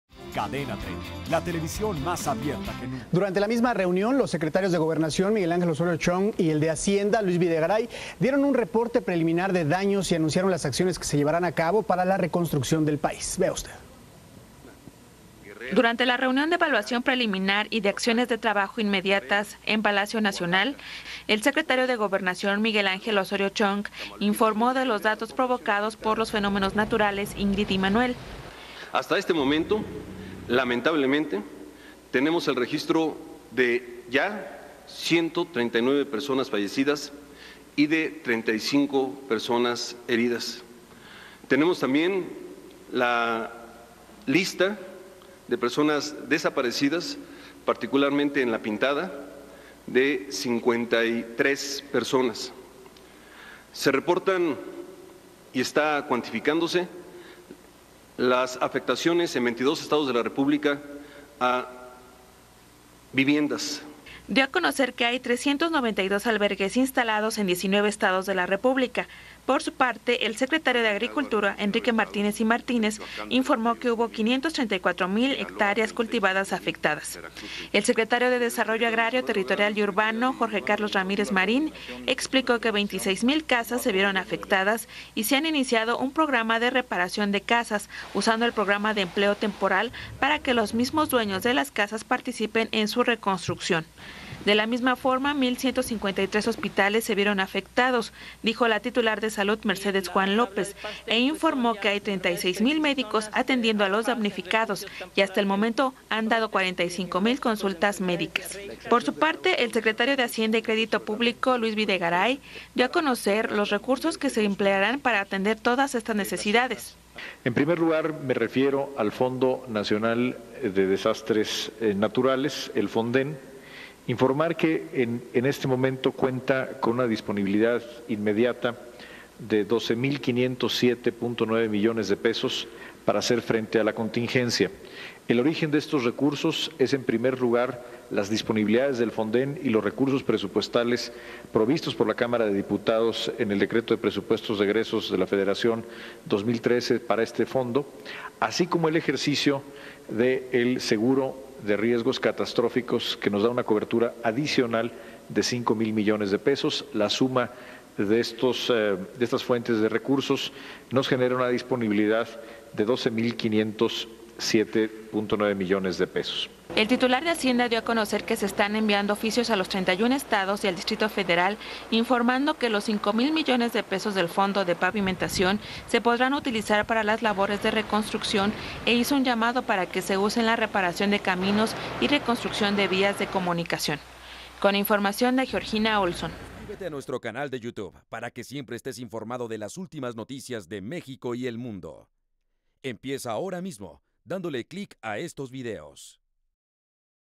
Osorio Chong y Luis Videgaray dan reporte preliminar de acciones para la construcción del país
26 Septiembre 2013

Los secretarios de gobernación Miguel Ángel Osorio Chong y de hacienda Luis Videgaray dan un reporte preliminar de daños y anuncian las acciones que se llevarán a cabo para la reconstrucción del país.